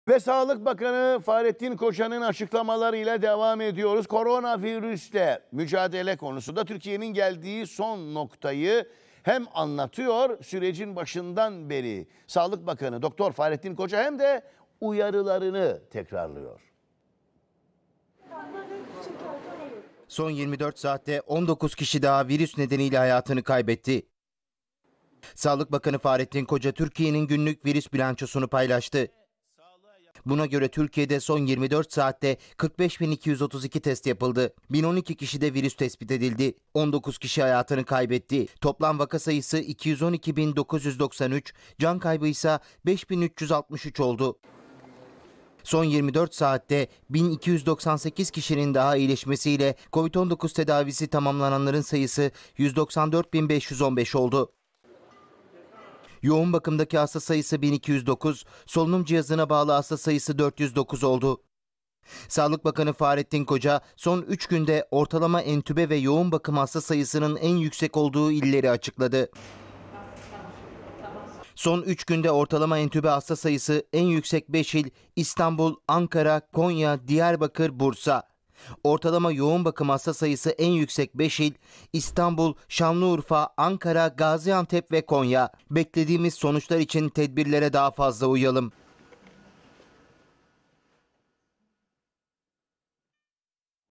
Sağlık Bakanı Fahrettin Koca Açıkladı : Koronavirüsten Bir Günde 19 Can Kaybı!
Türkiye'de son 24 saatte 1012 kişiye yeni tip koronavirüs (Kovid-19) tanısı konuldu, 19 kişi hayatını kaybetti, toplam vaka sayısı 212 bin 993, can kaybı 5 bin 363 oldu.

Sağlık Bakanlığının internet sitesinde yer alan ve Bakan Fahrettin Koca'nın da Twitter'dan paylaştığı "Türkiye Günlük Koronavirüs Tablosu"nun güncel verilerine göre, bugün 45 bin 232 test yapıldı, 1012 kişiye Kovid-19 tanısı konuldu. Son 24 saatte 19 hasta vefat etti, 1298 kişi iyileşti.